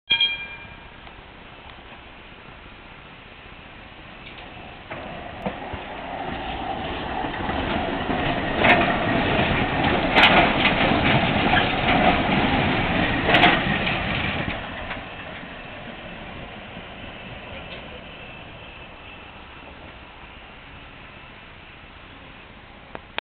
Düwag GT8 ZR #904, wachadłowa linia W, pętla Piątkowska, Poznań
Tramwaj typu GT8 ZR na tymczasowej linii wachadłowej W : Piątkowska - Wielkopolska. Dnia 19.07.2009r